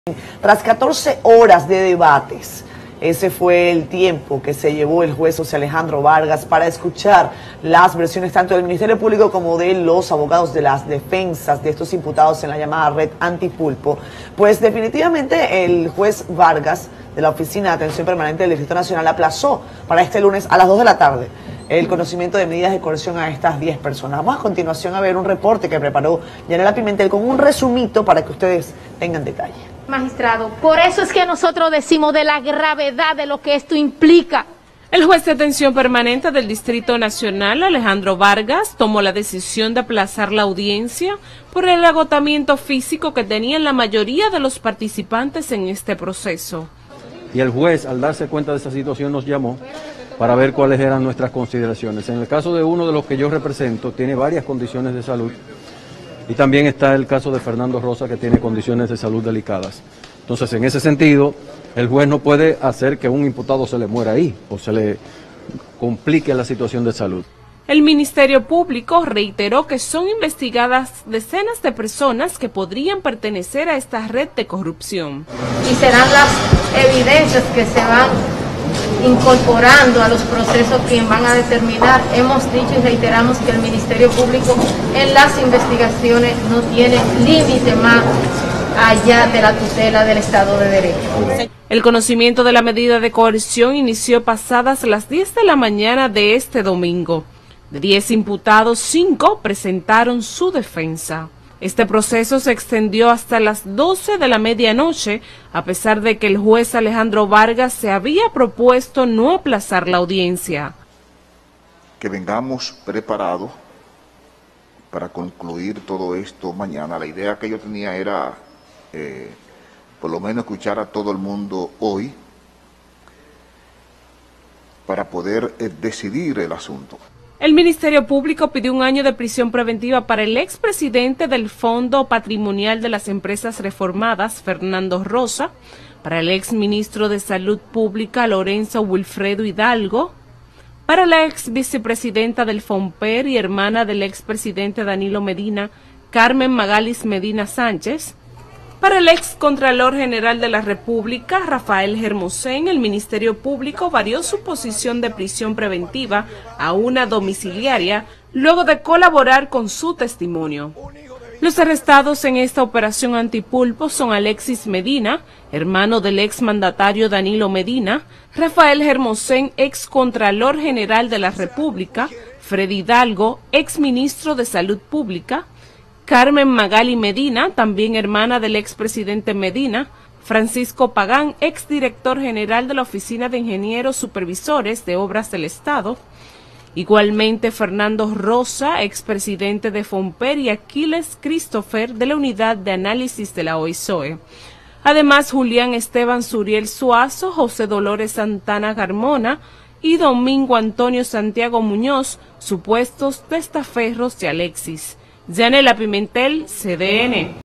Conocimiento de medidas de coerción a implicados en “Operación Anti-Pulpo” continúa este lunes
Santo Domingo RD.- El juez José Alejandro Vargas, de la Oficina Judicial de Servicios de Atención Permanente del Distrito Nacional, aplazó para este lunes a las 2:00 de la tarde la audiencia de medidas de coerción contra los once imputados del pasado gobierno acusados de actos de corrupción administrativa, entre los que destacan dos hermanos del expresidente Danilo Medina, arrestados la semana pasada por presuntamente aprovechar sus vínculos con el poder político para acumular fortunas millonarias.

El magistrado tomó la decisión pasadas las 12 de la media noche.

El Ministerio Público estuvo representado por los procuradores adjuntos Wilson Camacho y Yeni Berenice Reynoso, de que los imputados operaban en un entramado de corrupción contra el Estado dominicano dirigido por Juan Alexis Medina.